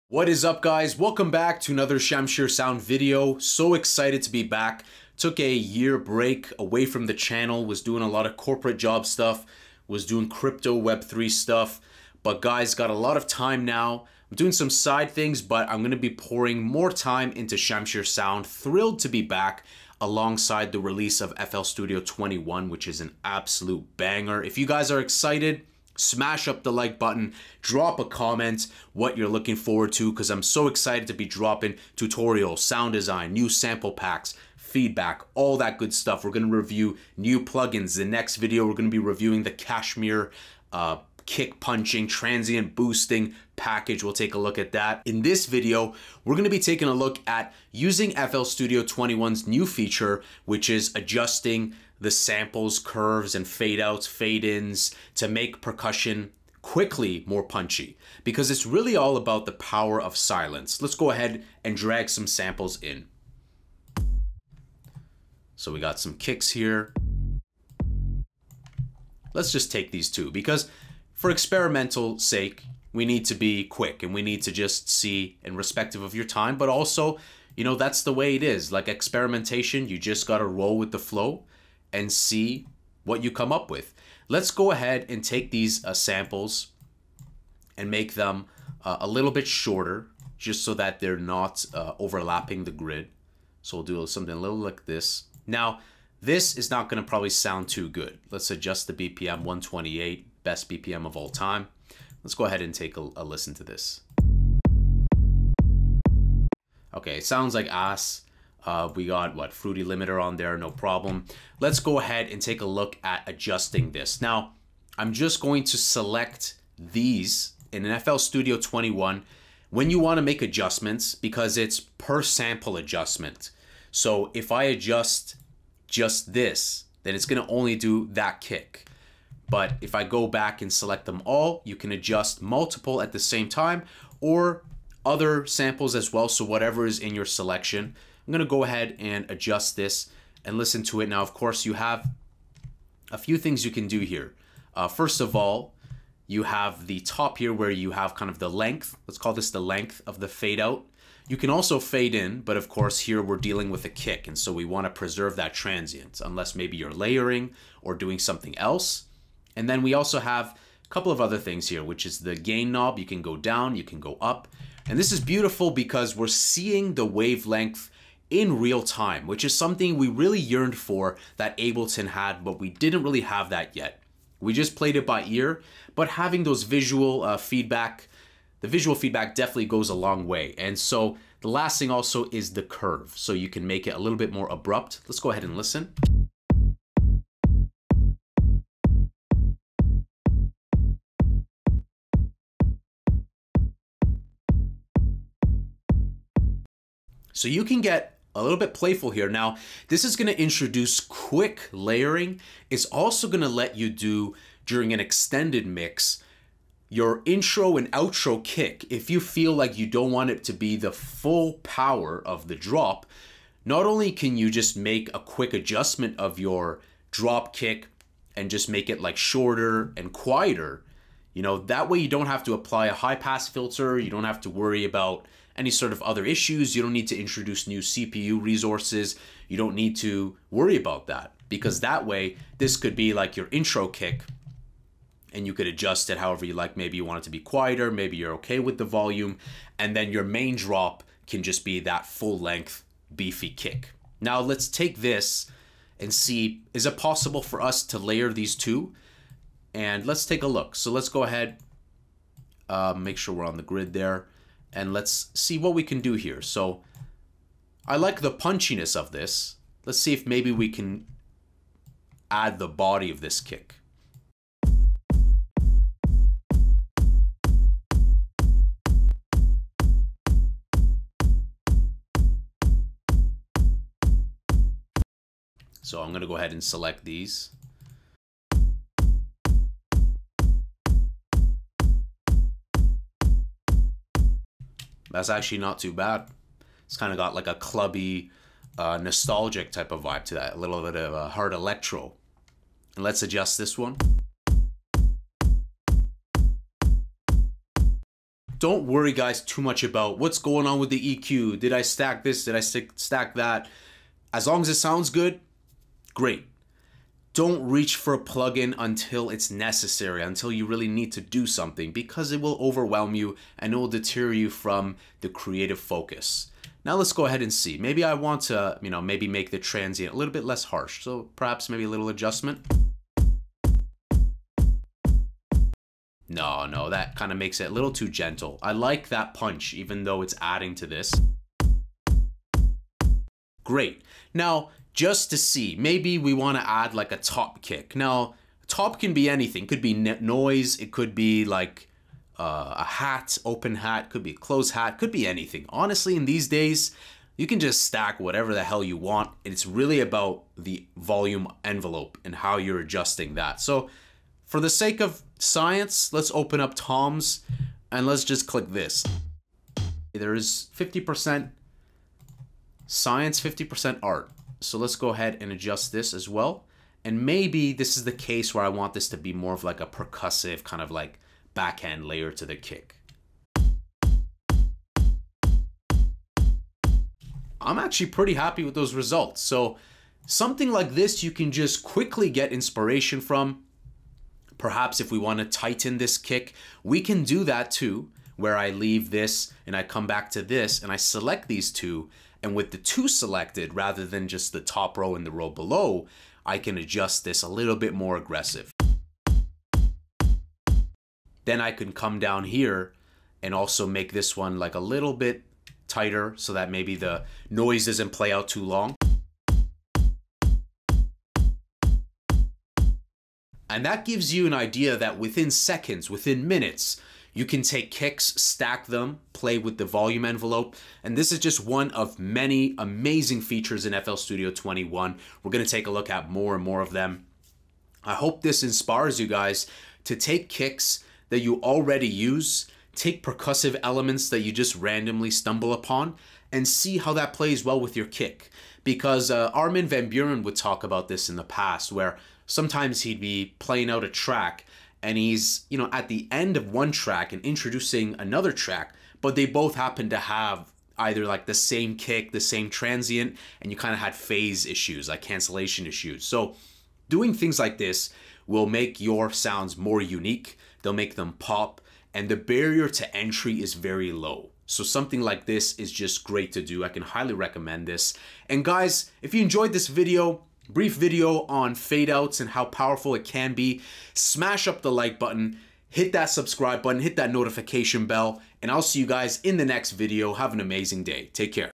Stacking Kicks Using FL Studio 21's Powerful Volume Shaping | Layering Techniques
Today we'll take a look at the new volume envelope shaping for waveforms on the playlist in FL Studio 21! This creates a ton of new possibilities for printed sidechain, trimmed kicks, stacked percussion, allowing more creativity, visual feedback, and less CPU resources!